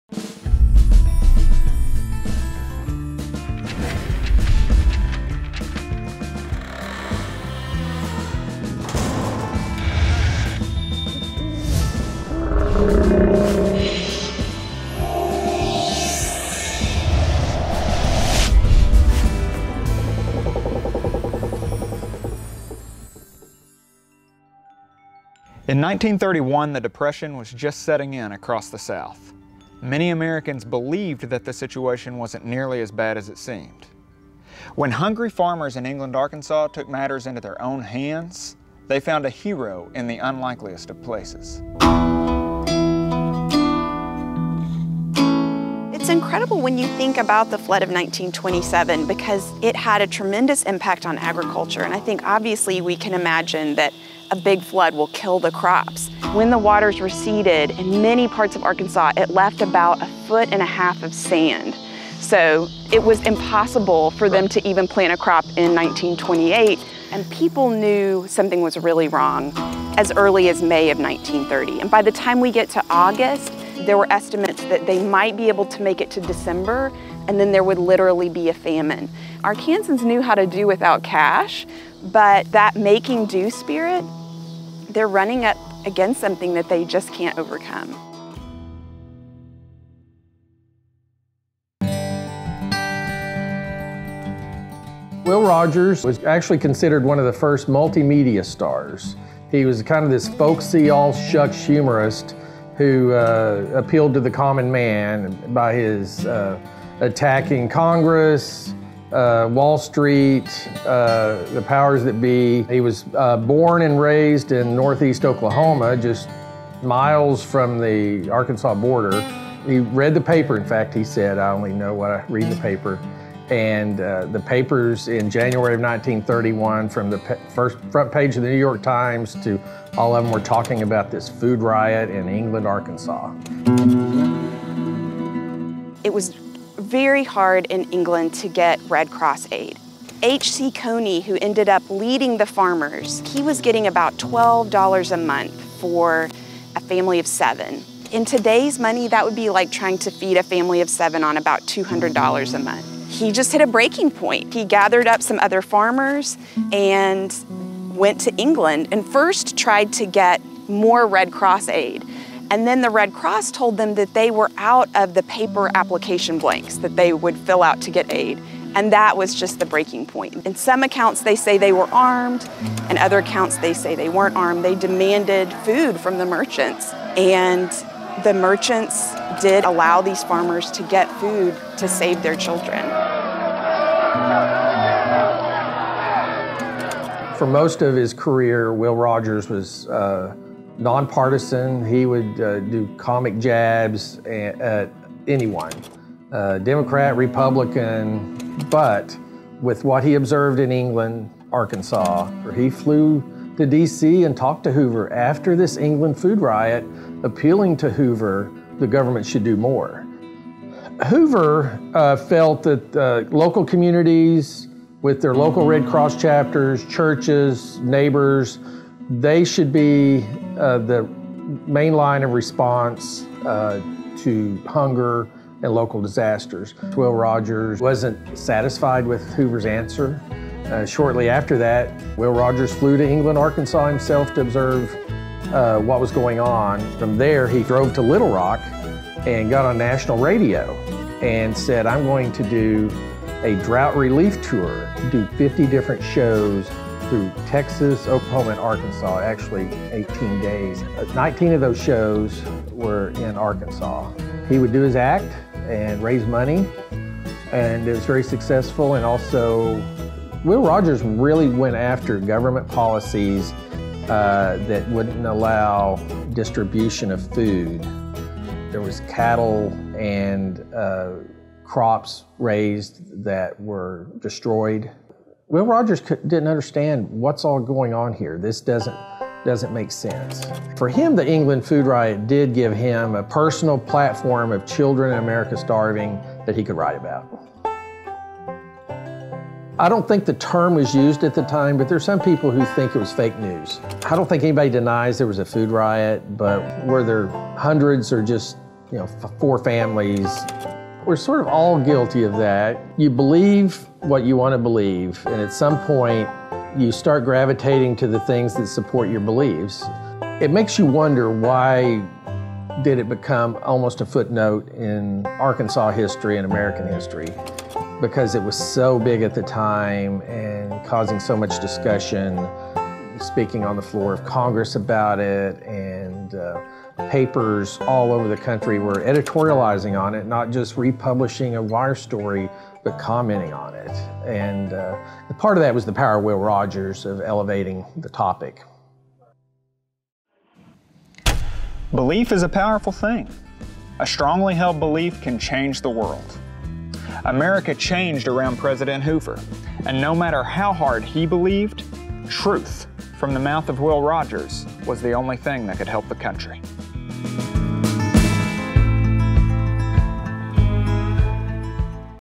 Once Upon a Time in Arkansas: Will Rogers Fights for England
1931 was a nightmare for England, Arkansas, farmers. Between the effects of 1927 flood and the Great Depression, their children were starving. What was worse? No one in D.C. was taking the problem seriously. Find out how a celebritytook on their cause and changed the nation’s perspective.